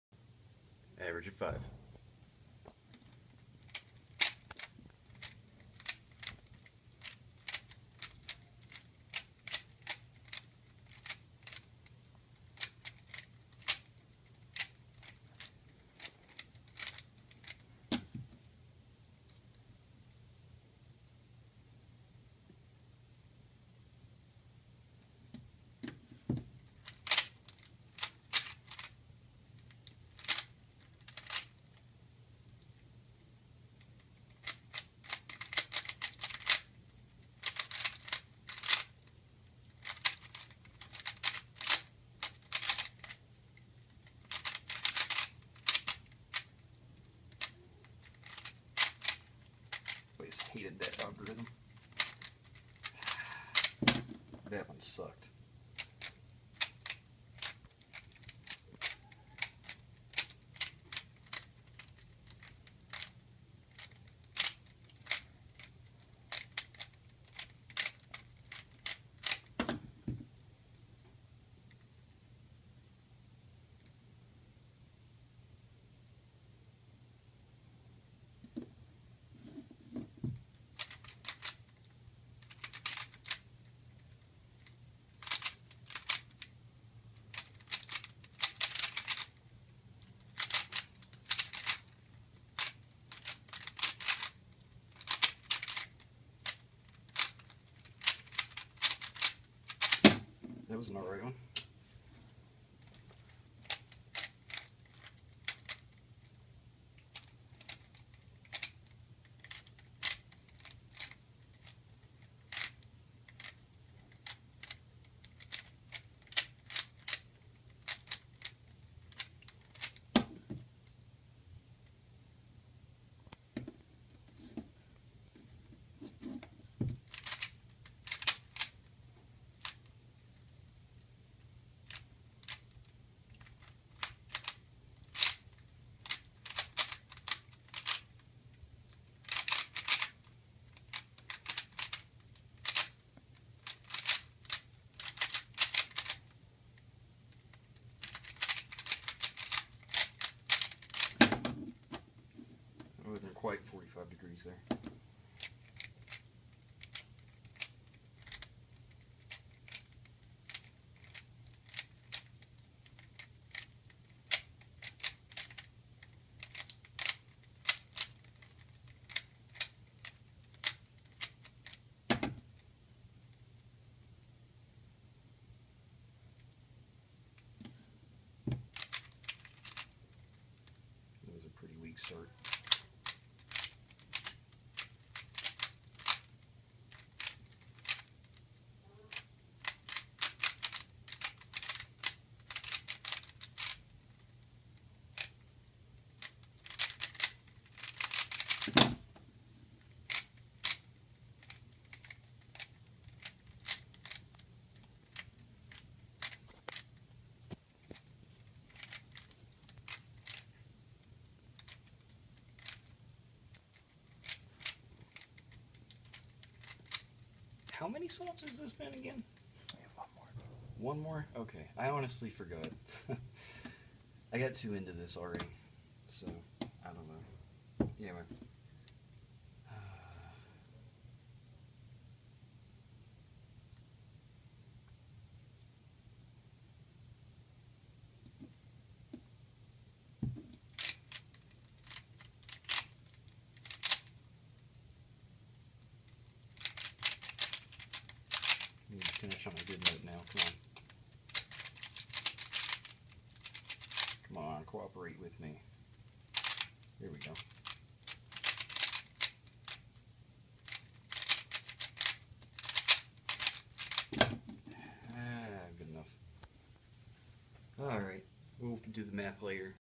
3x3x3 Rubik's Cube - 23.20 Average of 5
Since it's been a while since my last 3x3x3 speed solve, I figured I'd post something. At the time of recording, my personal best was 15.77 seconds. These were some of the first solves I did with this cube, and probably slowed me down just a bit.